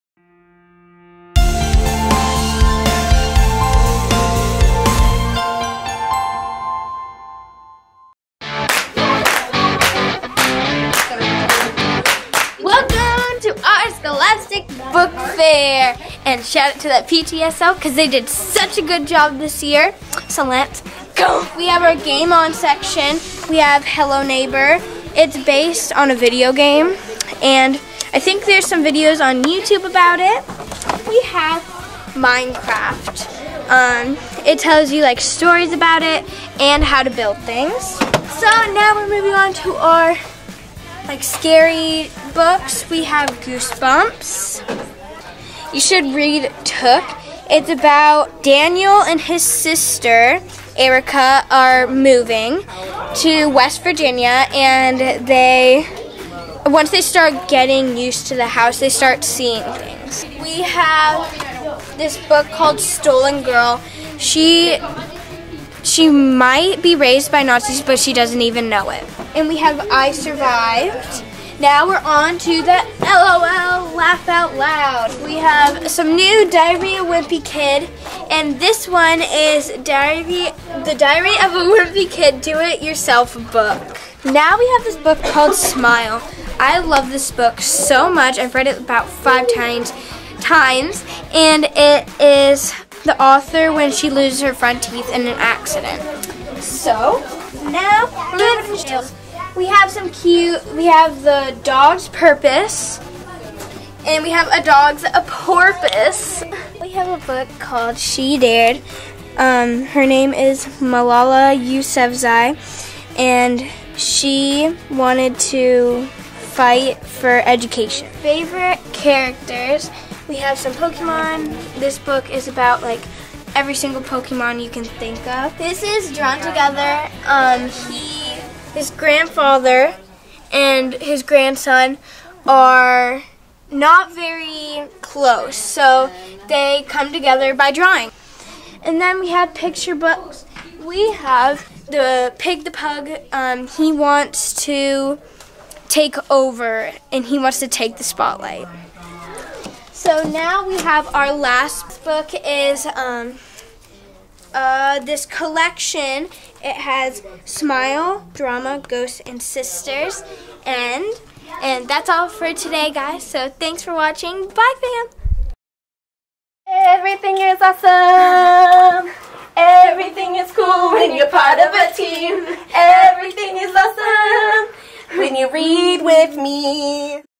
Spring Scholastic Book Fair Tour! || Boxes of Books
Links to all the books can be found below!

Sydney gives us a fabulous Spring Scholastic Book Fair Tour! You'll find new gems and past favorites!

Missing Pieces (Hello Neighbor #1)
Chasing Herobrine: An Unofficial Graphic Novel for Minecrafters, #5:
Dogman Series Set
Haunted Halloween: Movie Novel (Goosebumps the Movie 2):
Took: A Ghost Story
Stolen Girl
I Survived Set
Diary of a Wimpy Kid Collection NEW 2018 Edition:
The Raina Telgemeier Collection (A Box Set)
Malala Yousafzai (She Dared)
A Dog's Purpose
A Dog's Porpoise
Drawn Together
Pig the Star (Pig the Pug)